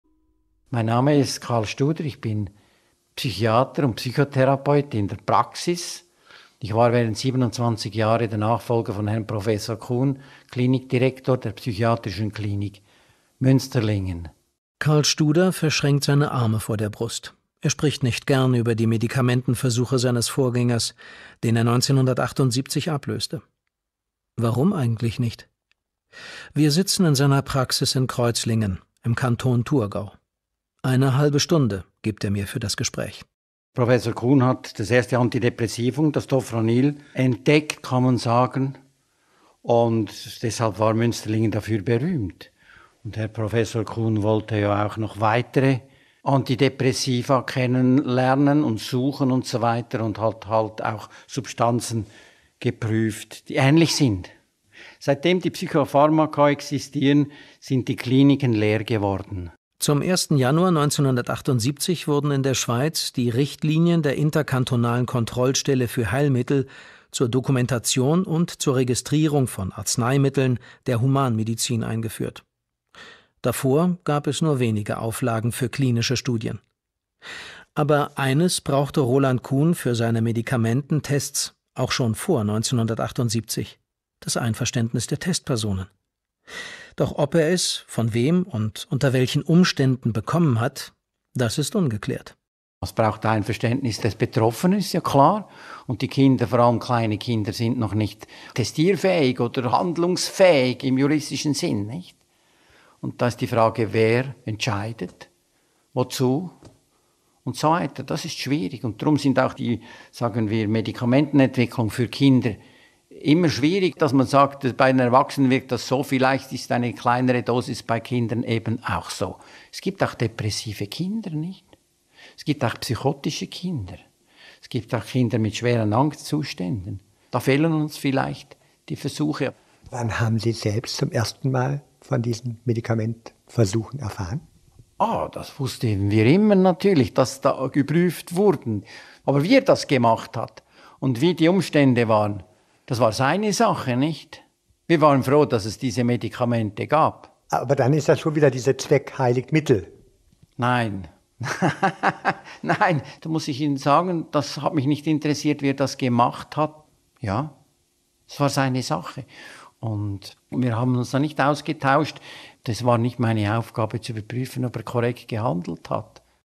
21.10.2014 Deutschlandfunk Ausschnitt mit Karl Studer
Deutschlandfunk
21. Oktober 2014
Ausschnitt mit Karl Studer
Karl Studer war 27 Jahre lang der Nachfolger von Professor Kuhn, Klinikdirektor der Psychiatrischen Klinik Münsterlingen TG von 1980 bis 2006.

Ausstellung von Walter Emmisberger
zum Thema fürsorgerische Zwangsmassnahmen, Medikamententests und Behördenwillkür in der Schweiz.